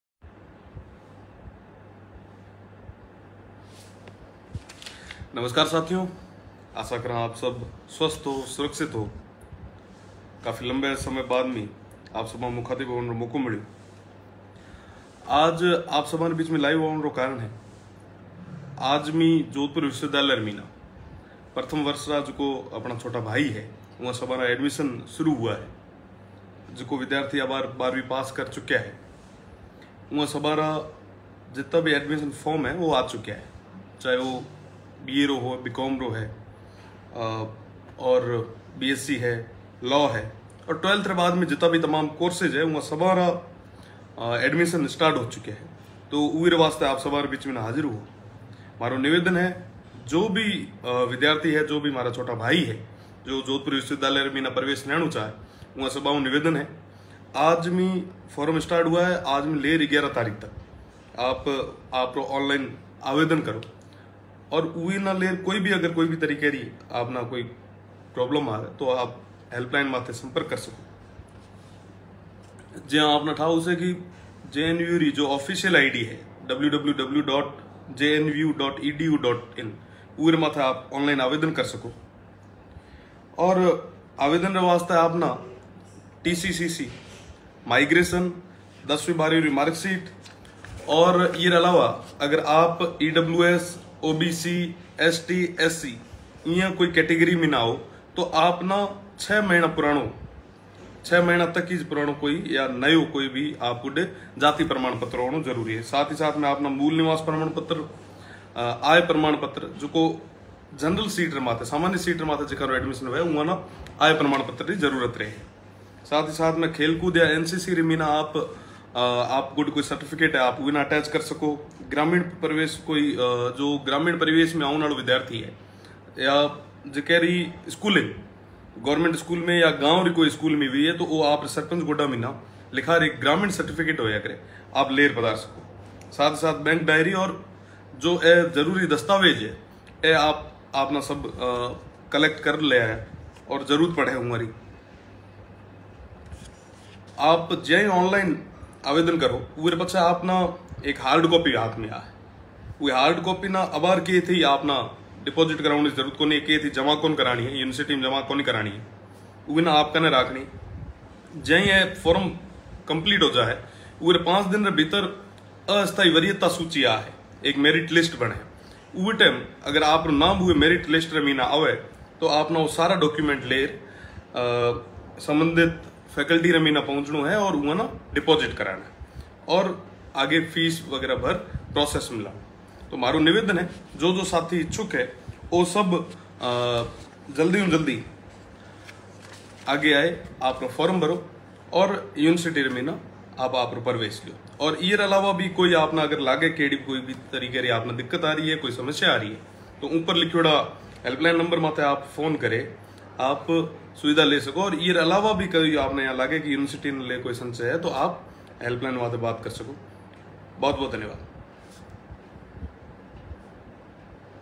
प्रथम वर्ष के प्रवेश संबंधित जानकारी
प्रथम वर्ष के प्रवेश प्रारम्भ हो चुके है जो विद्यार्थी जोधपुर विश्वविद्यालय में प्रवेश लेना चाहते है वो पर ऑनलाइन फ़ॉर्म भर सकते है इसकी अंतिम तारीख़ 11 जुलाई है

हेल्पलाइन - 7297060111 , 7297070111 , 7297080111